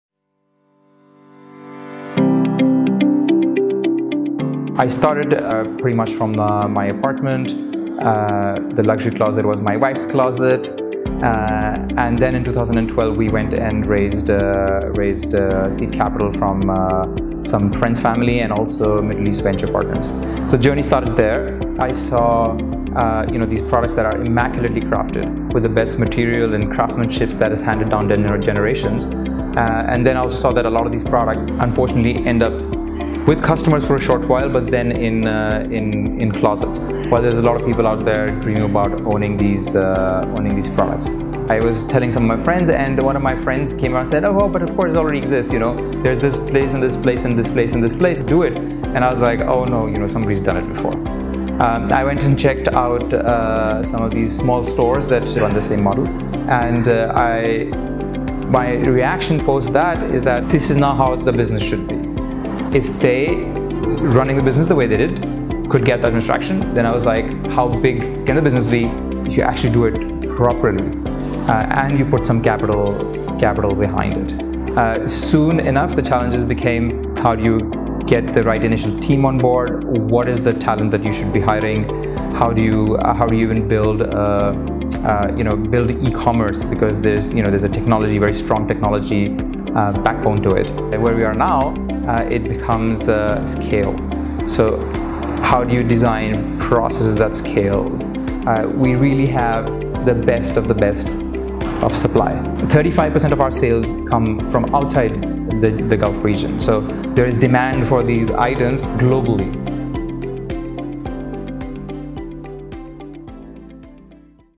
Luxury Link: In Conversation With The Luxury Closet Founder Kunal Kapoor
The Luxury Closet Founder Kunal Kapoor talks to Forbes Middle East about how he built one of the biggest online marketplaces for pre-owned luxury goods in the region and how he plans to scale the company.